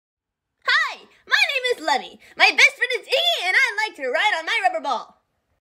Berryrrst's voice test
Here's a voice audition from @Berryrrst for my new "show"